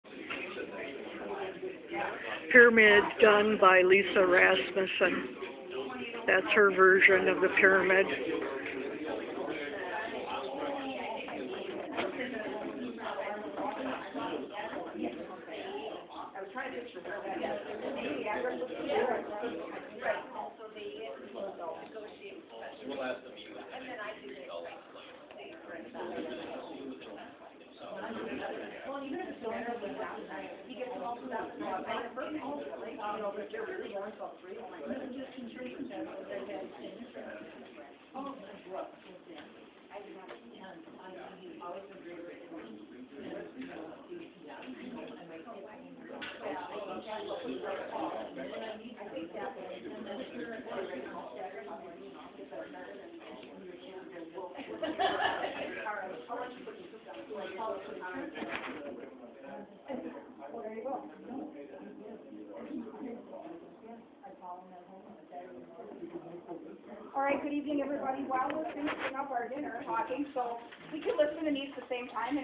City Training 01
May 9, 2016  city of Wausau alderpersons training with Mayor Rob Mielke and Council President Lisa Rasmussen with the following alderpersons in attendance:  Pat Peckham; Dave Nutting; Dennis Smith; Becky McElhaney; Joe Jehin; Gary Gesselman; Sherry Abitz; Karen Kelbach;  Tom Neal; and Romey Wagner.